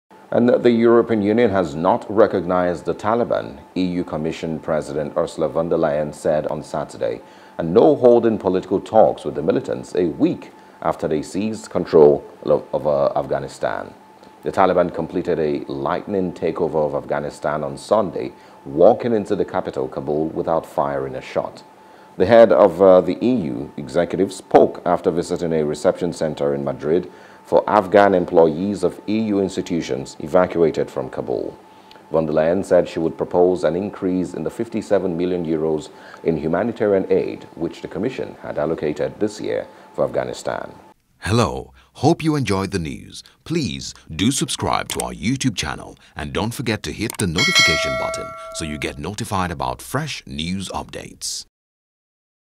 E.U On Afghanistan: No Recognition Of Taliban, No Political Talks | FOREIGN
The European Union has not recognised the Taliban, EU Commission President Ursula von der Leyen said on Saturday. And no holding political talks with the militants, a week after they seized control of Afghanistan.